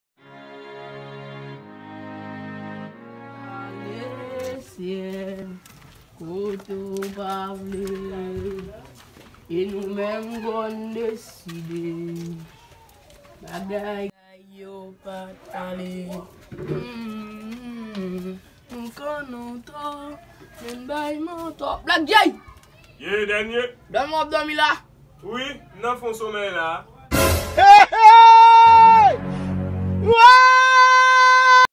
gad jan yon nèg sòt mezanmi oooooooo😥😥😥😥😥😥
Dènye ap pale ak Black G epi li mandel sise dòmi l ap dòmi la ?